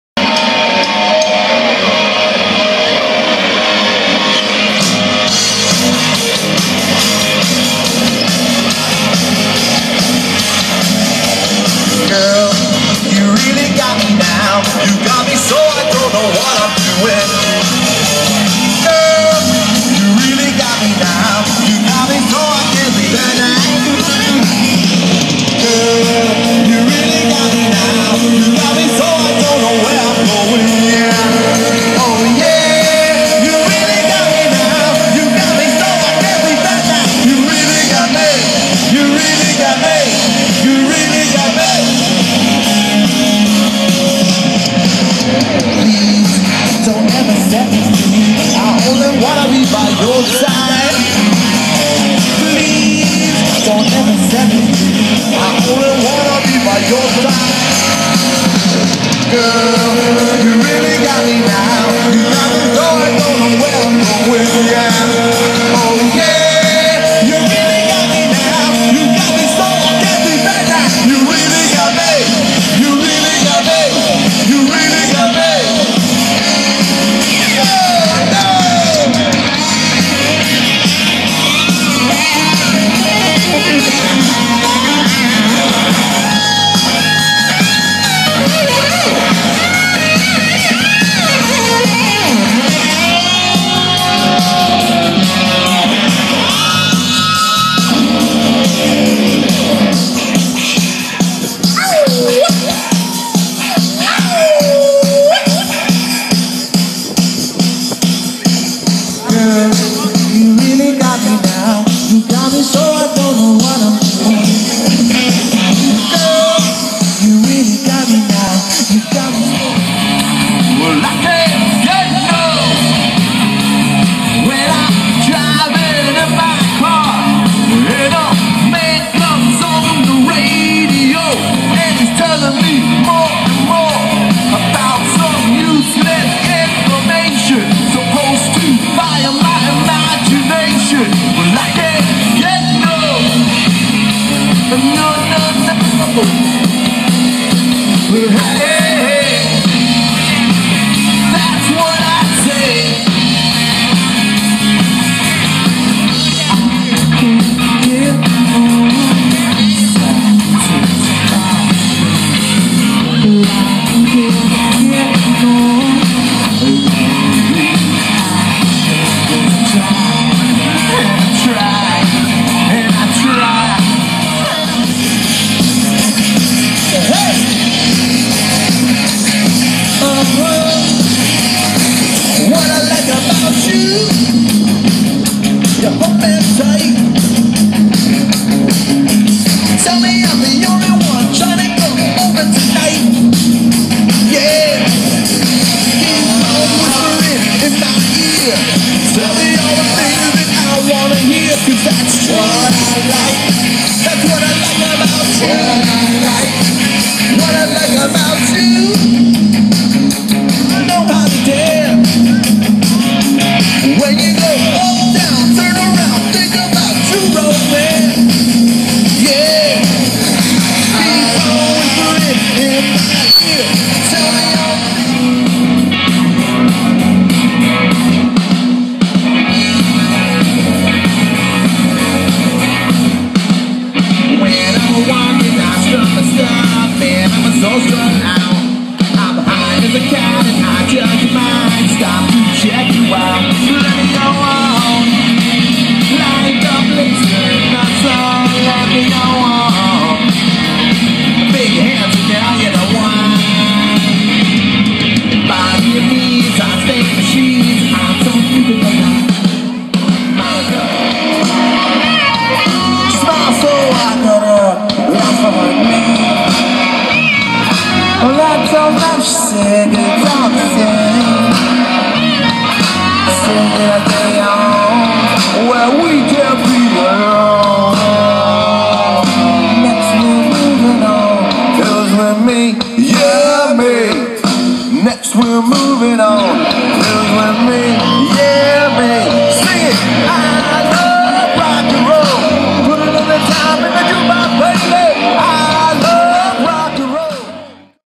Hard Liquor Band playing at the Warrior Dash event
Hard Liquor Band playing at the Warrior Dash event in Miami Florida